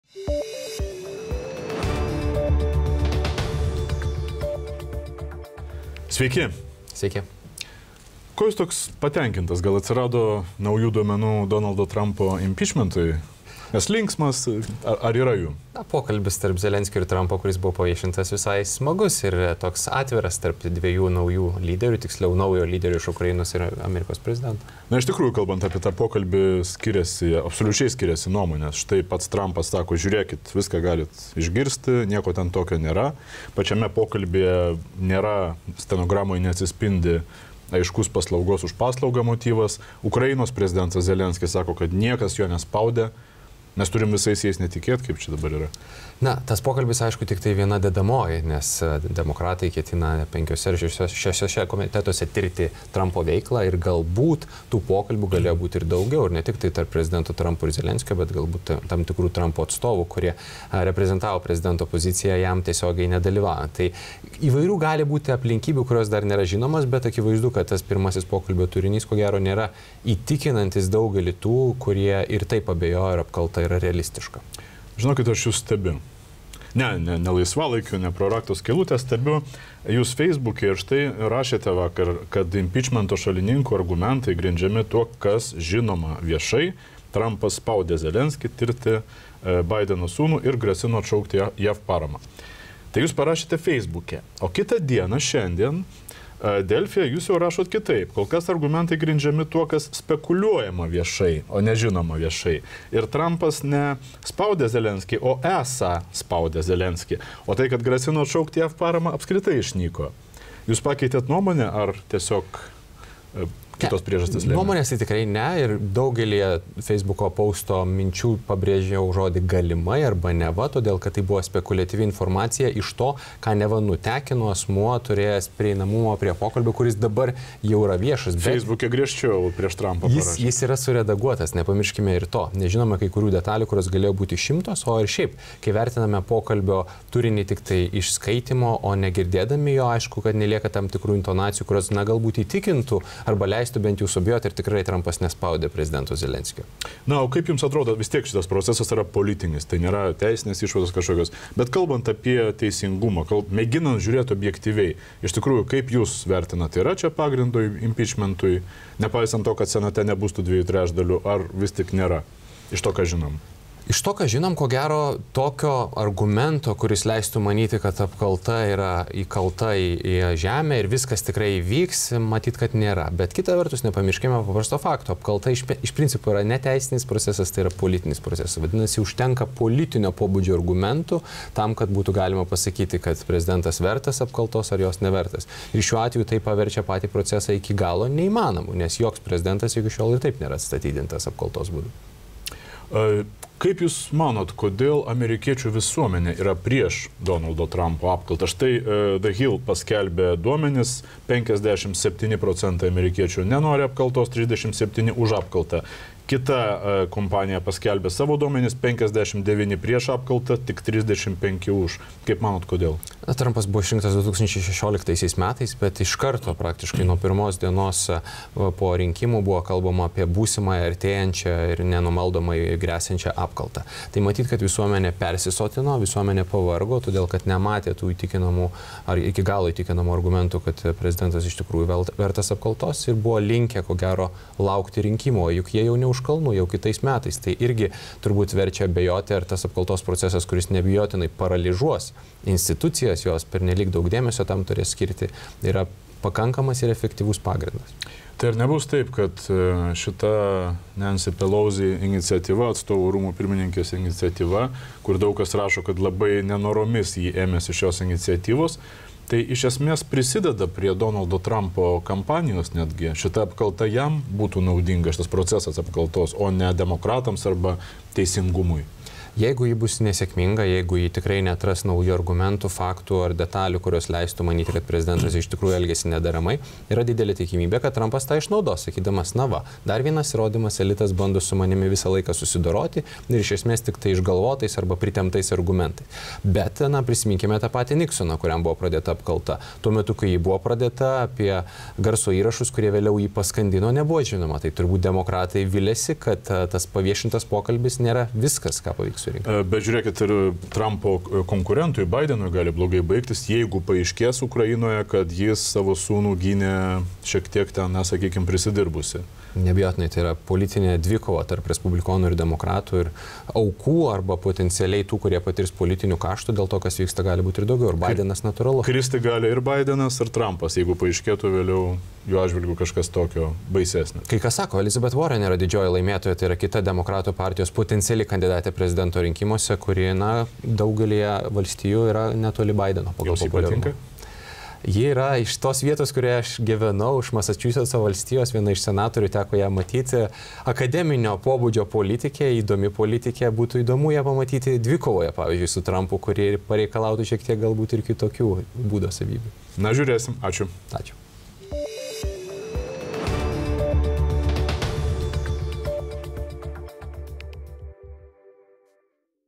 Linas Kojala: iš to, ką žinome, argumentų Trumpo apkaltai nėra
Paviešinus prezidento Donaldo Trumpo ir Ukrainos vadovo Volodymyro Zelenskio telefoninio pokalbio stenogramą Amerikoje kilo skandalas. Demokratai inicijuoja apkaltą Trumpui, tačiau ar yra jai pagrindo?
Apie tai „Dienos komentare“ kalbamasi su Rytų Europos studijų centro vadovu Linu Kojala.